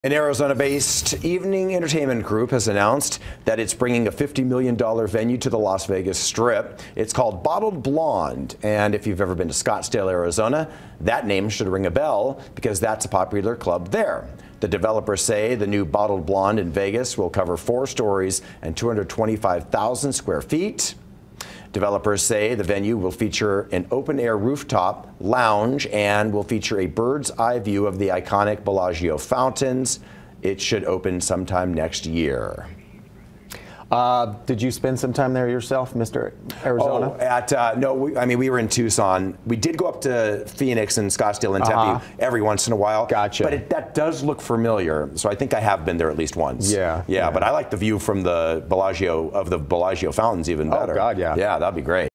Arizona-based group to bring $50M venue to Las Vegas Strip
An Arizona-based group has announced that it will bring a $50 million venue to the Las Vegas Strip.